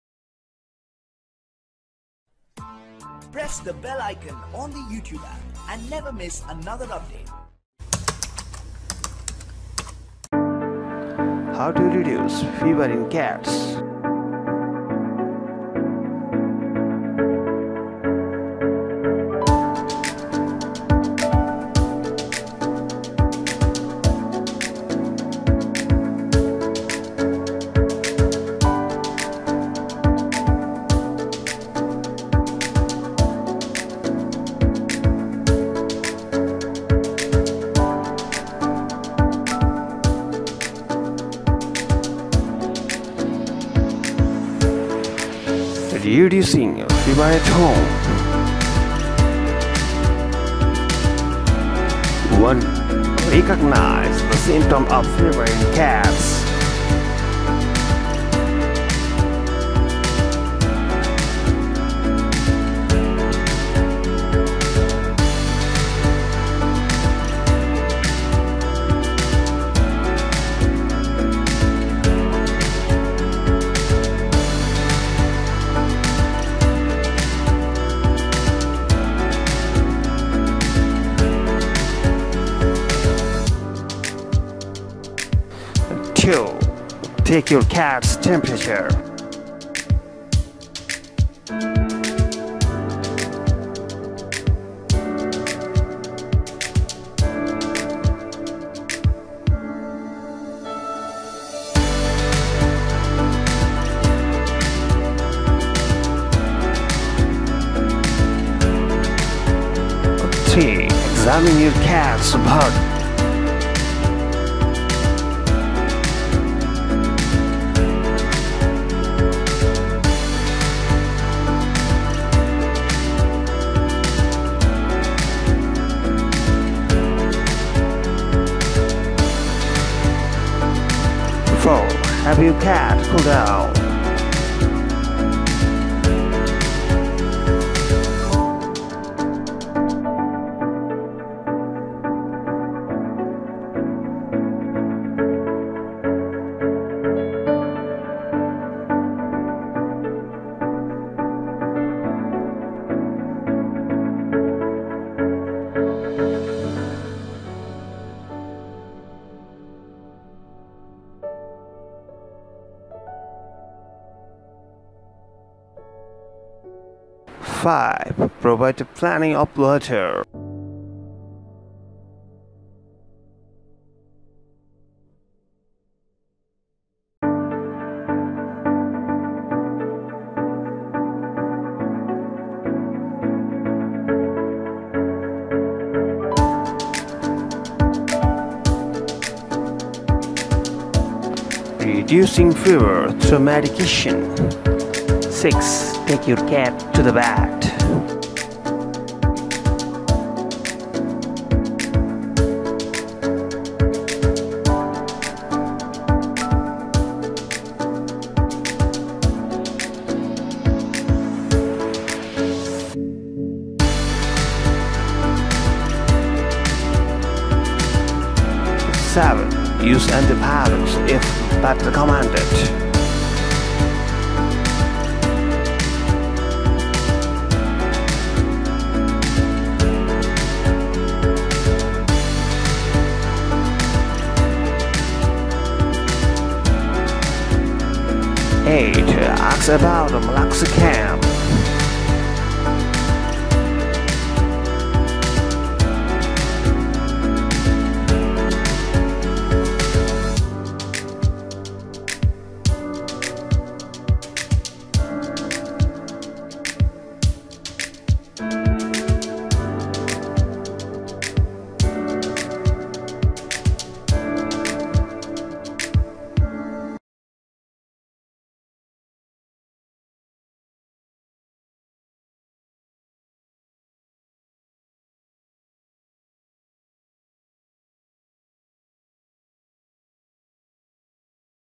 How to Reduce Fever in Cats Steps By Steps ! Cat Health Tips 2021
Whether it’s you or your cat, having a fever isn’t necessarily a bad thing. It’s a normal immune response that helps the body recover from illness by killing heat-sensitive bacteria. Making your cat more comfortable will help her get over her fever and back to normal.

in this  video we provide How to Reduce Fever in Cats at Home.

► How to Help a Dog Produce More Milk for Her Puppies 2020  ! Dog Health Tips 2021